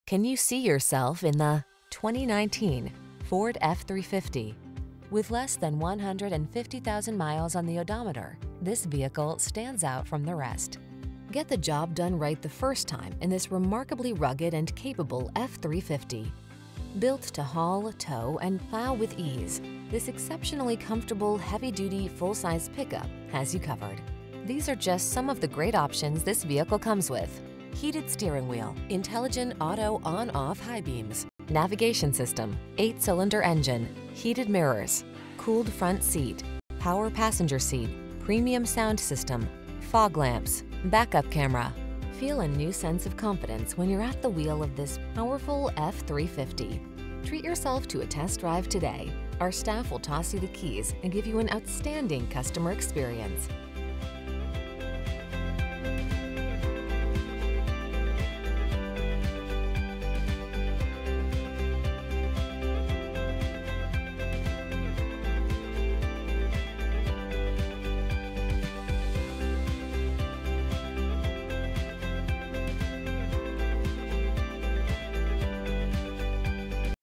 2019 Ford Super_Duty_F-350_SRW King Ranch Hattiesburg, Jackson, Biloxi, Mobile, Sumrall MS
Pre-Owned 2019 Ford Super_Duty_F-350_SRW King Ranch available at Toyota of Hattiesburg located in 6461 US Hwy 98, Hattiesburg, MS, 39402.

Super_Duty_F-350_SRW

Can you see yourself in the  2019  FORD F-350.  Get the job done right the first time in this remarkably rugged and capable F-350. Built to haul, tow, and plow with ease, this exceptionally comfortable heavy-duty, full-size pickup has you covered. These are just some of the great options this vehicle comes with: Heated Steering Wheel
, Intelligent Auto on/off High Beams, Navigation System, 8 Cylinder Engine, Heated Mirrors, Cooled Front Seat(s), Power Passenger Seat, Premium Sound System, Fog Lamps, Back-Up Camera. Feel a new sense of confidence when you're at the wheel of this powerful F-350. Treat yourself to a test drive today. Our staff will toss you the keys and give you an outstanding customer experience.